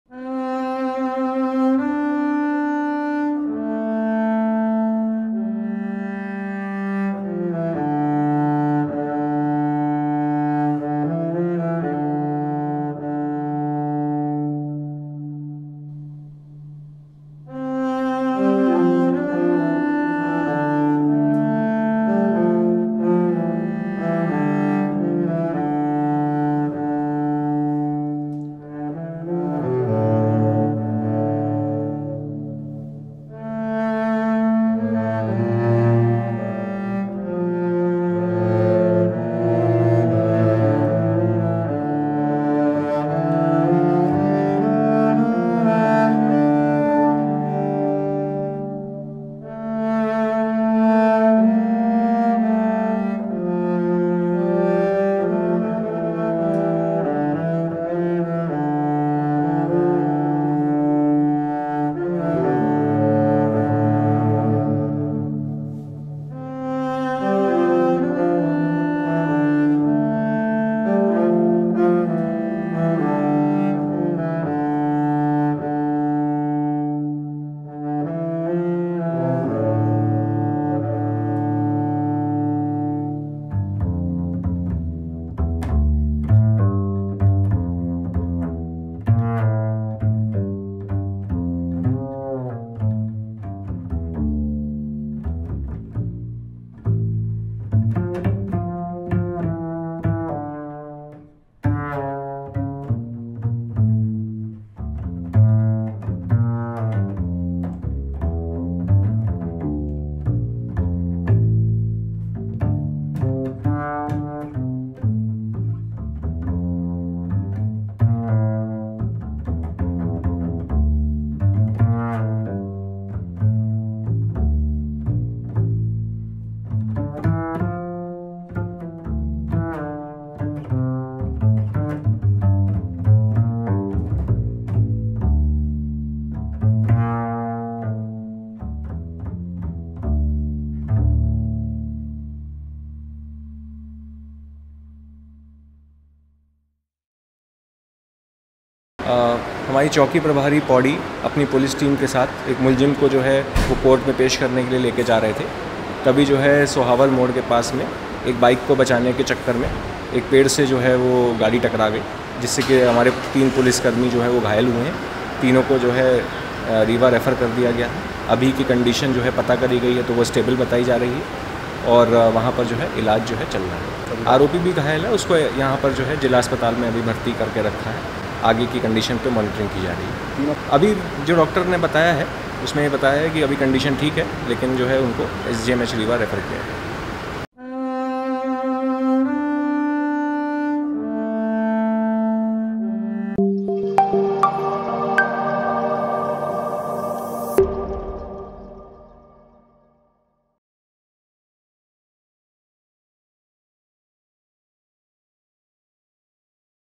Satna News: नशे कारोबारी को कोर्ट ले जाते समय पेड़ से टकराई गाड़ी, बाइक सवार को बचा रहा था ड्राइवर!
सतना सोहावल में पुलिस वाहन अनियंत्रित होकर पेड़ से टकरा गई. जिसमें 3 पुलिस कर्मी और एक अपराधी घायल हो गए. एनडीपीएस के आरोपी को नागौद थाना की पौड़ी चौकी से न्यायालय ले जा रहे थे, तभी रास्ते में सभी लोग दुर्घटना का शिकार हो गए. इस घटना में TI अरुण त्रिपाठी, SI पंकज मिश्रा और कांस्टेबल क्रांति मिश्रा घायल हुए. घटना के बाद सभी को सतना जिला अस्पताल इलाज के लिए लाया गया. हालत नाज़ुक होने की वजह से सभी को रीवा SGMH रेफर किया गया. जहां पर इलाज के दौरान कांस्टेबल क्रांति मिश्रा की मौत हो गई.